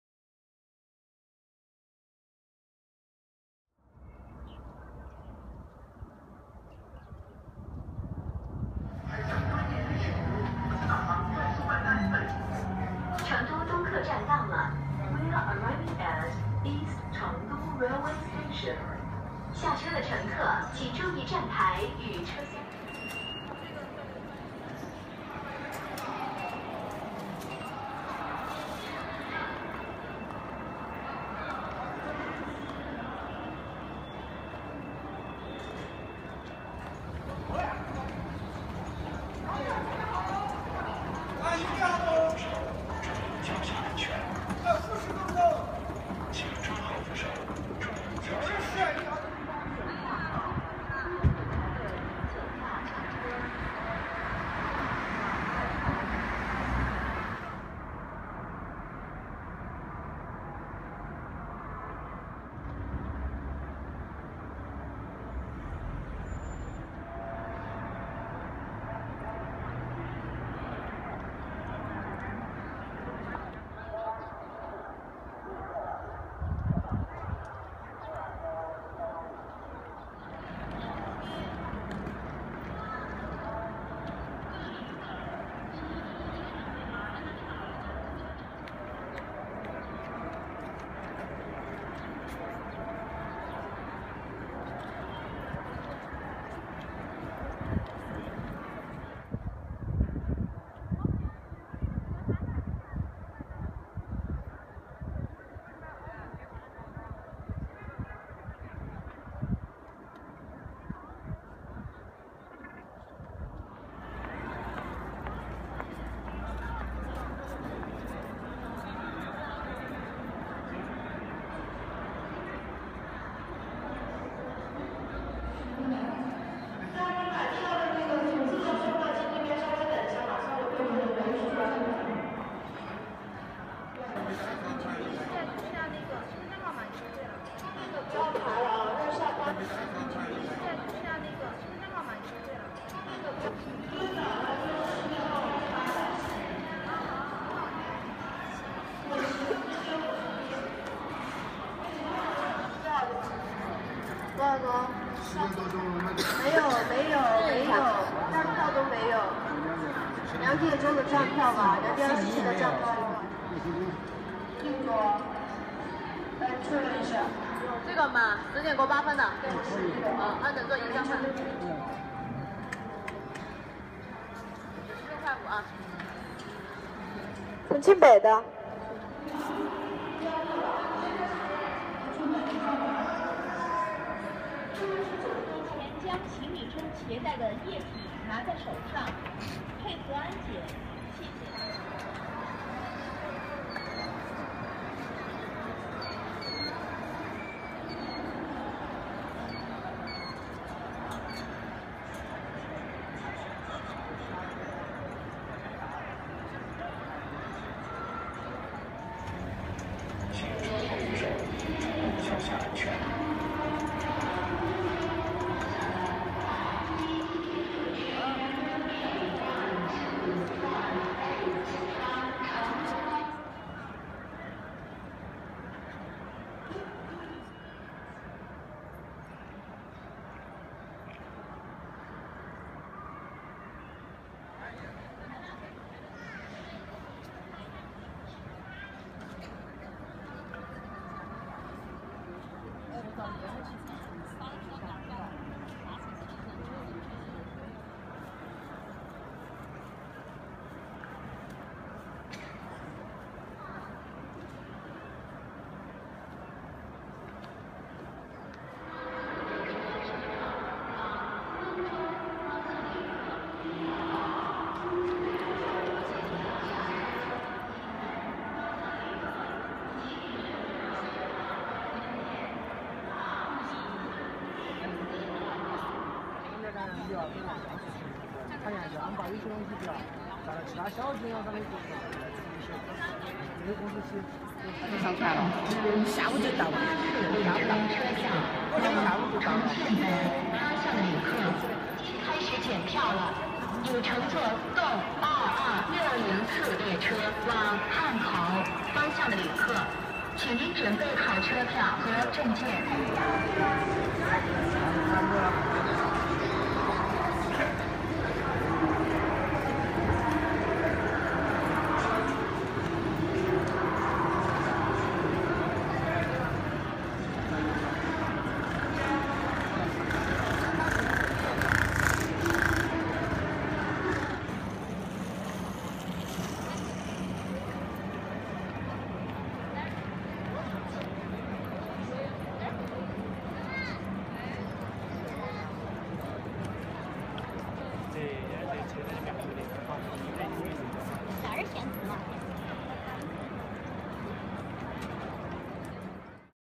Chengdu East Railway Station Guide - departure
The Chengdu East Railway station is large and modern. Watch this video to learn you way around this station before even going there.